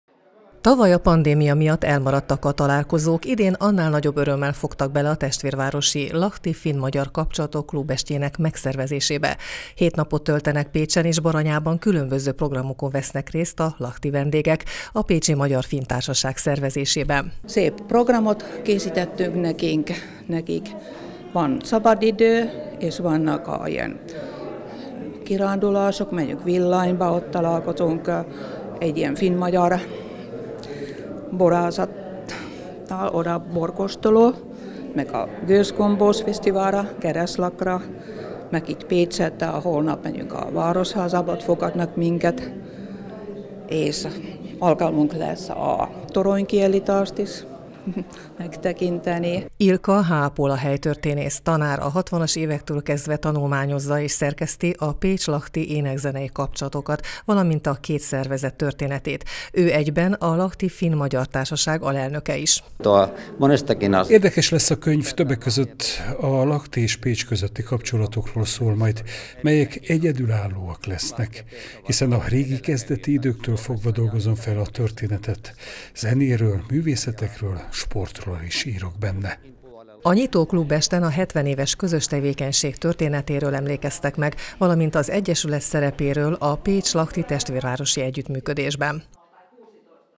70 éves Lahti Finn–Magyar Társaság - hét napos rendezvénysorozat a finn és magyar vendégeknek
70 éves Lahti Finn–Magyar Társaság - hét napos rendezvénysorozat a finn és magyar vendégeknek

A pécsi finnbarátok 1974-ben alakult közössége és finn testvérszervezetük, az  idén 70 éves Lahti Finn–Magyar Társaság 1977 óta szervez kölcsönös cserelátogatásokat. Az általában nyolc-tíznapos látogatás során  mindkét városban az együttműködő egyesületek és a vendégeket otthonukban fogadó házigazdák szervezésében gazdag program várja őket. Közös klubest, városnézés, múzeumlátogatás, gasztronómiai és zenei programok, folklórest, nyelvgyakorlás színesíti a találkozó napjait. Szerdán este klubesttel indult a programsorozat, melynek a Civil Közösségek Háza adott otthont.